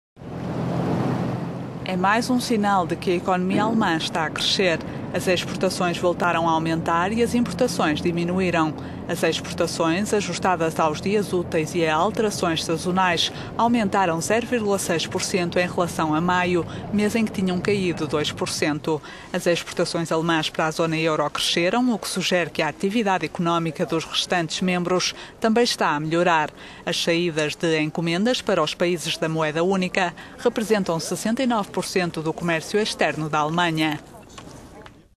Exportações alemãs voltam a aumentar - economy
É mais um sinal de que a economia alemã está a crescer. As exportações voltaram a aumentar e...

É mais um sinal de que a economia alemã está a crescer. As exportações voltaram a aumentar e as importações diminuíram.

As exportações, ajustadas aos dias úteis e a alterações sazonais, aumentaram 0,6% em relação a maio, mês em que tinham caído 2%.